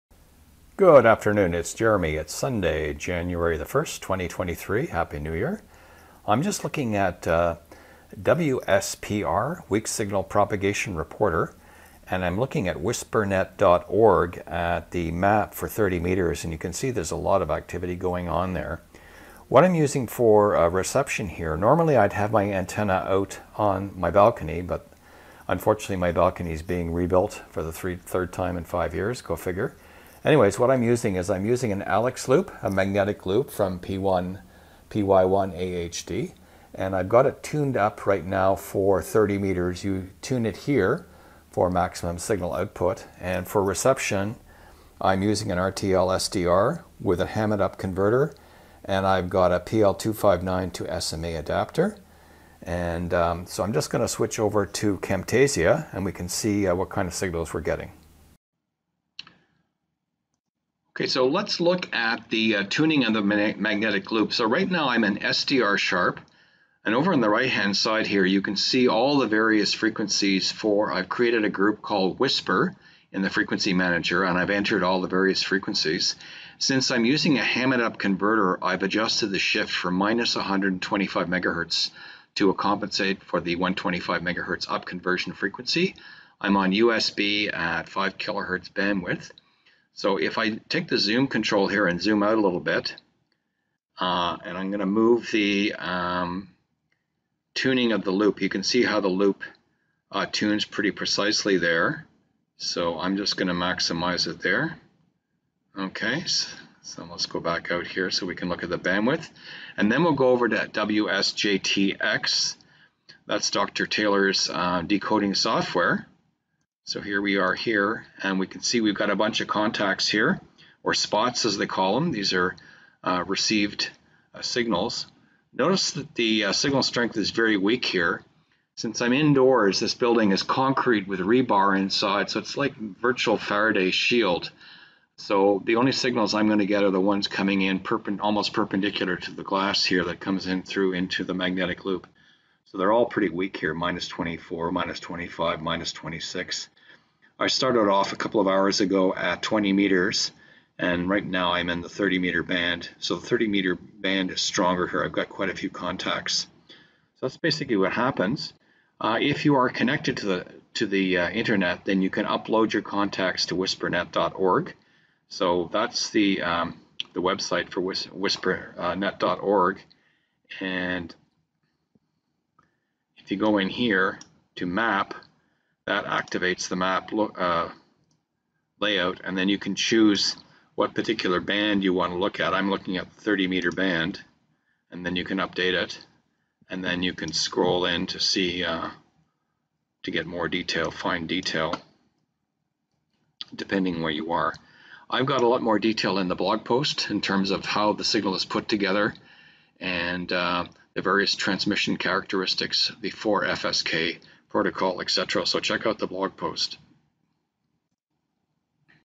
RTL-SDR for WSJT-X WSPR
The RTL-SDR can be used to decode WSPR Weak Signal Propagation Reporter signals. WSPR gives real time propagation status from actual amateur radio transmissions. Signal strengths can be as low as -28dB! Details in the BlogPost.